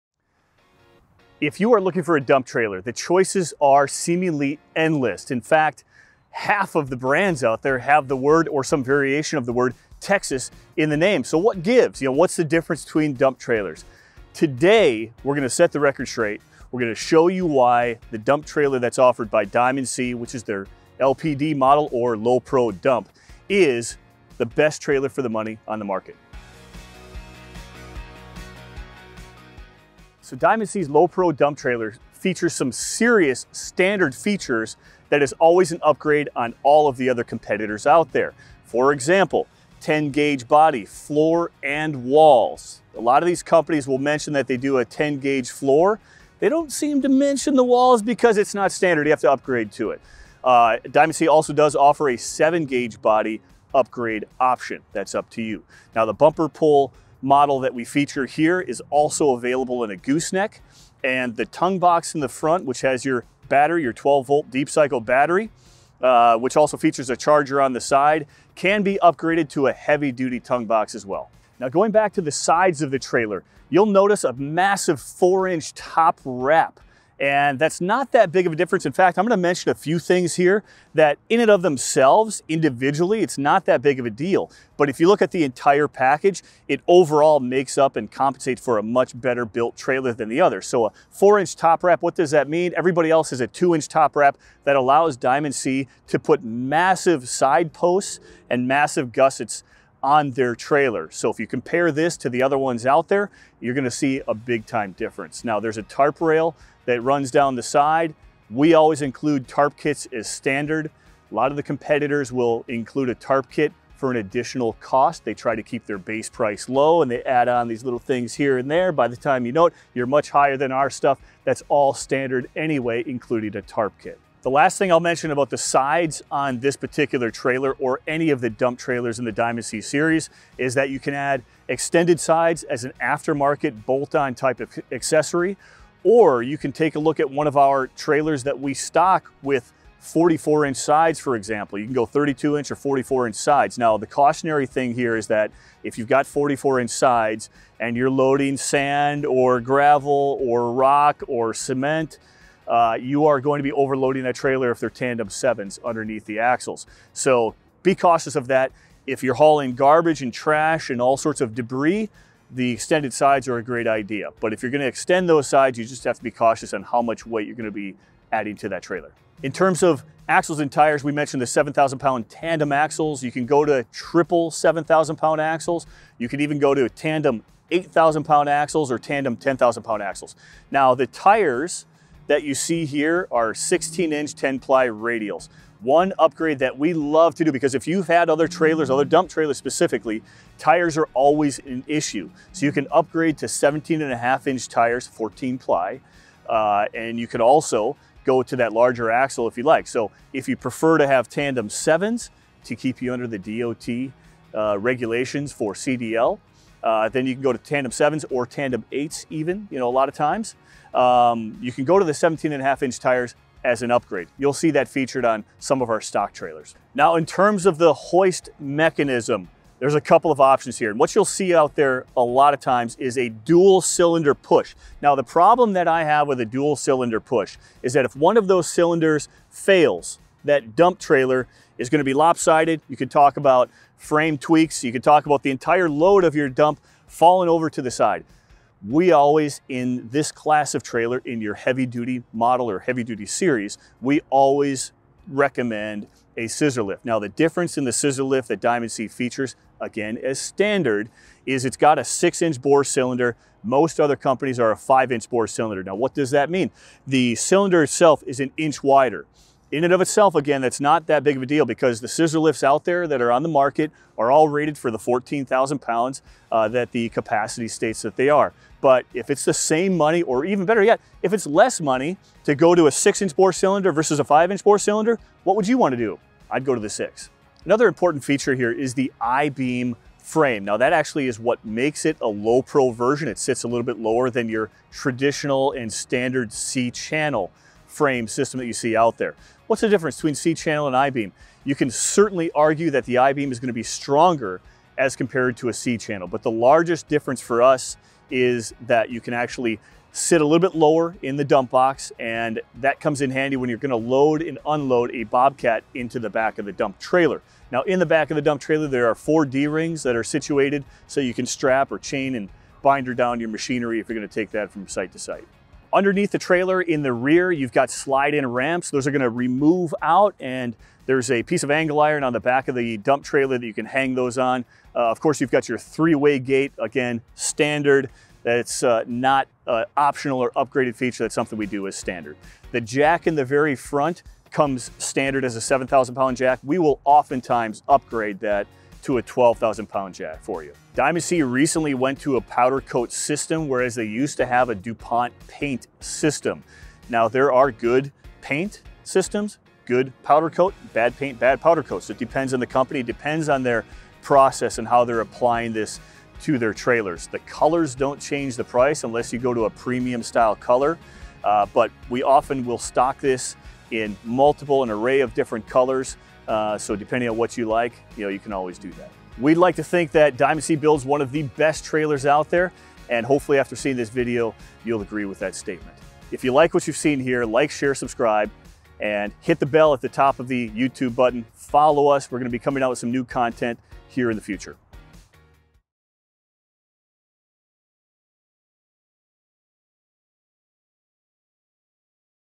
See why THIS is the best "low profile" dump trailer on the market for the price
Diamond C offers one of the best built dump trailers that money can buy. The entire trailer has been reinforced starting with the I-Beam frame, the heavy duty side posts, and of course the 6" cylinder scissor lift.
You might spend a little more than the "other guys" but you can count on this trailer for years to come.